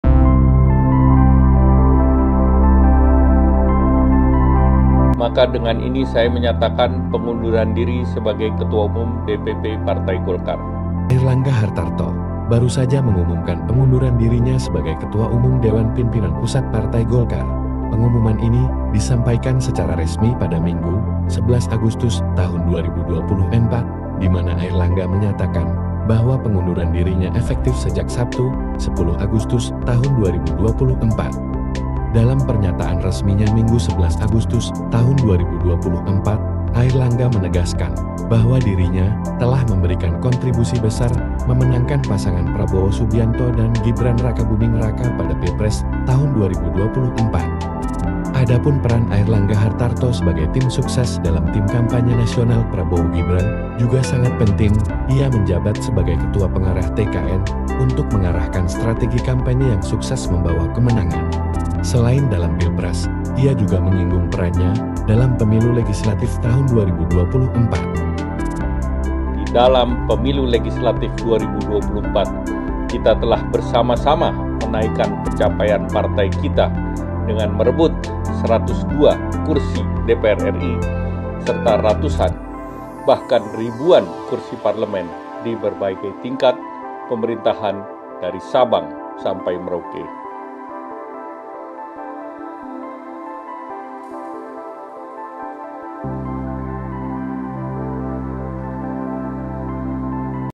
Mundur dari Ketum Golkar, Ini Kontribusi Besar Airlangga Hartarto
VIVA - Airlangga Hartarto, baru saja mengumumkan pengunduran dirinya sebagai Ketua Umum Dewan Pimpinan Pusat Partai Golkar. Pengumuman ini disampaikan secara resmi pada Minggu, 11 Agustus 2024, di mana Airlangga menyatakan bahwa pengunduran dirinya efektif sejak Sabtu, 10 Agustus 2024.

Dalam pernyataan resminya Minggu 11 Agustus 2024, Airlangga menegaskan bahwa dirinya telah memberikan kontribusi besar dalam memenangkan pasangan Prabowo Subianto dan Gibran Rakabuming Raka pada Pilpres 2024.

[RP-KY] {NAT}

Reporter : -
Kontributor : -